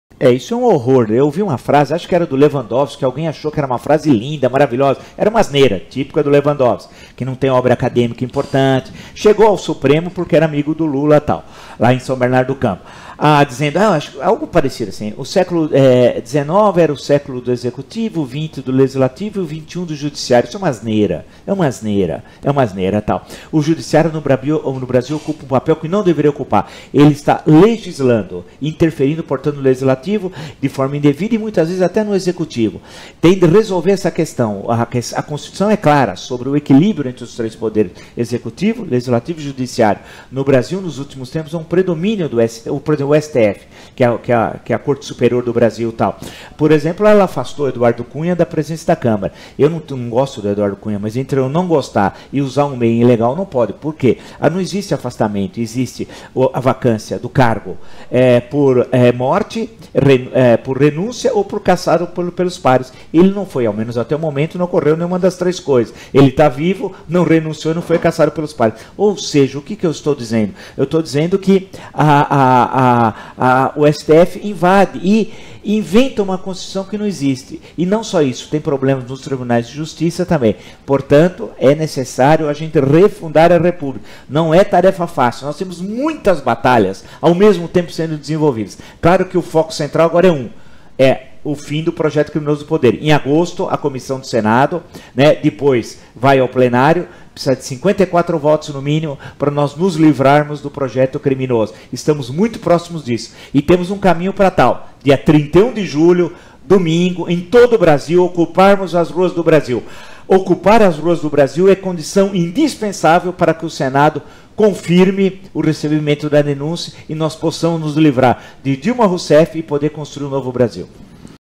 Marco Antonio Villa critica o Poder Judiciário comandado por Ricardo Lewandowski
“Corromperam o Brasil numa expectativa de um projeto de transformar isso aqui numa Cuba, numa Venezuela. Perderam feio. Agora acabou”. (Roberto Jefferson)